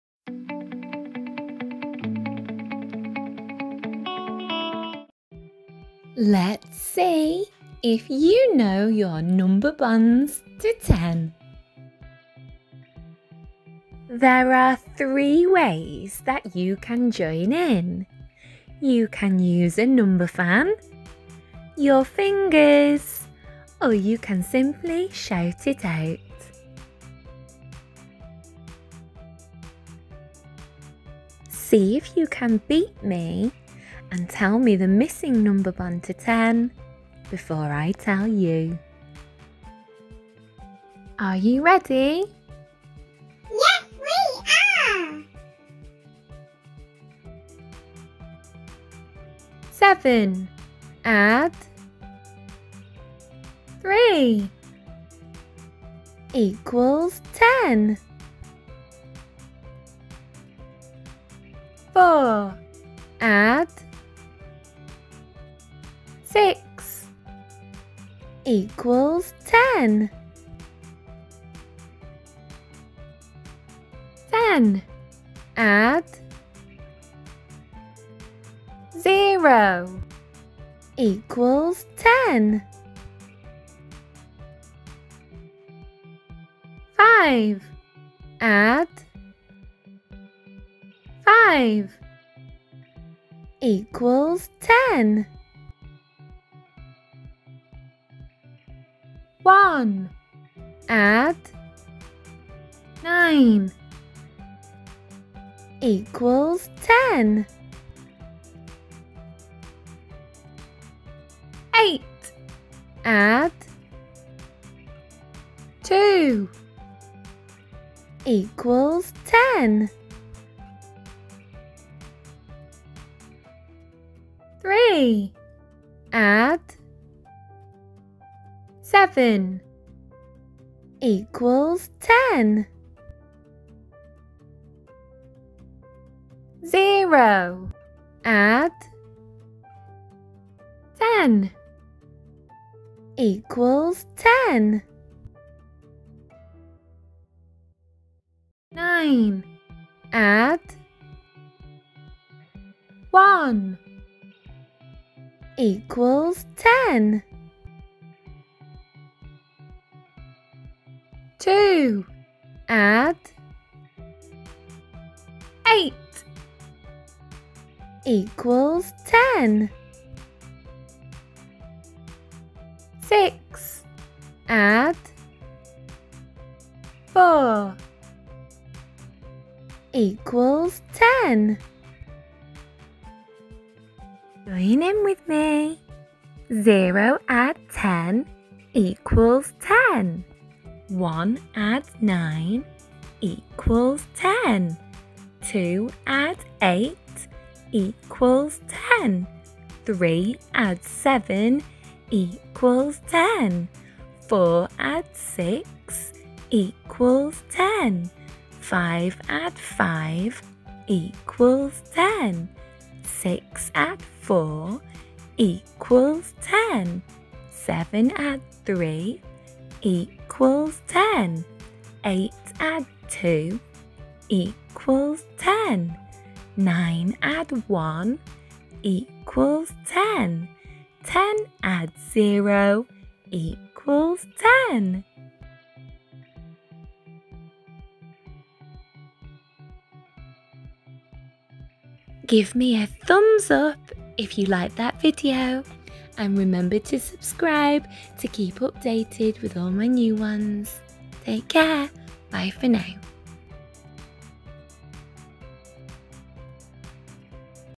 Numicon Number Bonds to 10 QUIZ | Learn Number Bonds to 10 | Miss Ellis 💜
Practise your number bonds to 10 using numicon by playing this fun quiz. You need to try to beat me by saying it first. Join in with a number fan, your fingers or simply by shouting it out. Learning your number bonds to 10 will really help to improve your maths skills so keep practising!